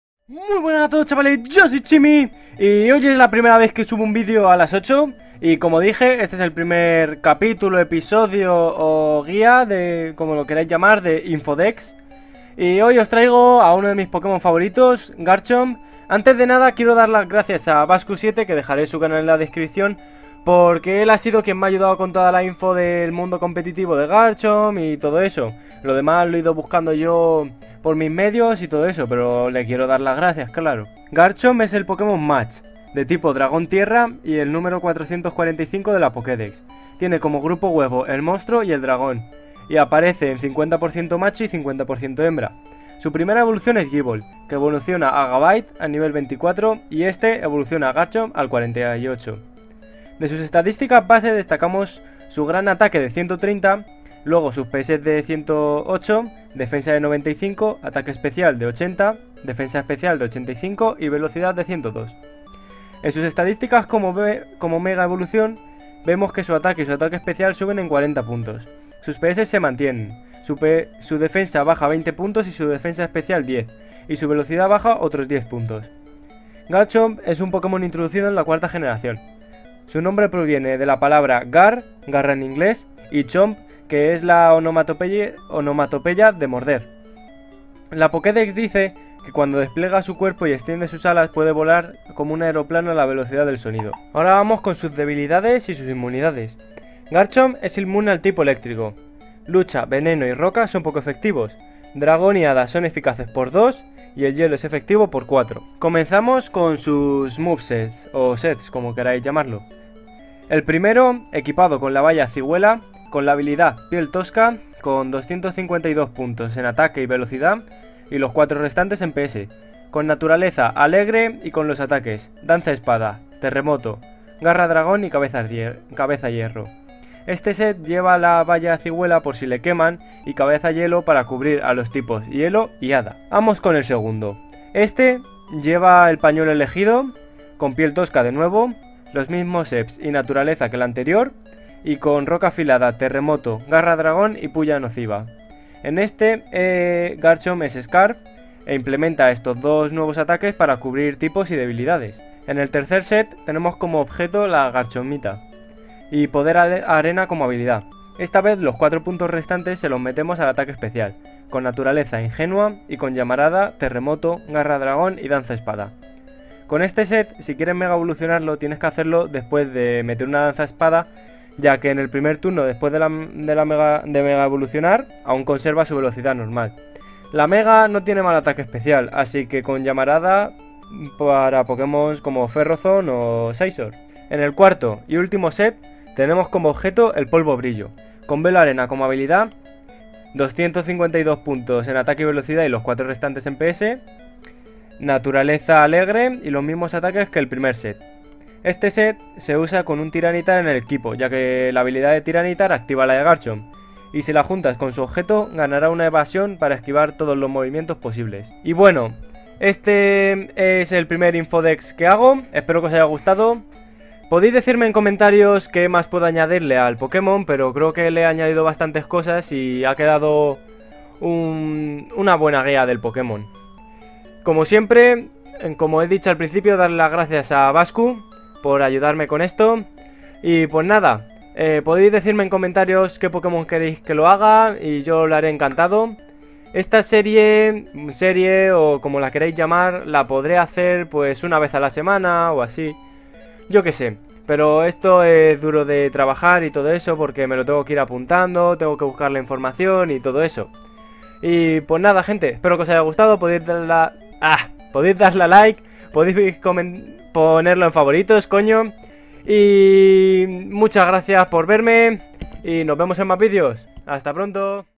InfoDex #01 | Garchomp
Buenas a todos chavales, yo soy Chiimy y hoy os traigo el primer InfoDex de mi canal, en el que hablo detalladamente del pokemon en cuestión, que en este caso es Garchomp, espero que os guste, en comentarios podéis darme ideas sobre otros pokemon de lo que hacerlo.

▼ Aquí tenéis todas mis series ▼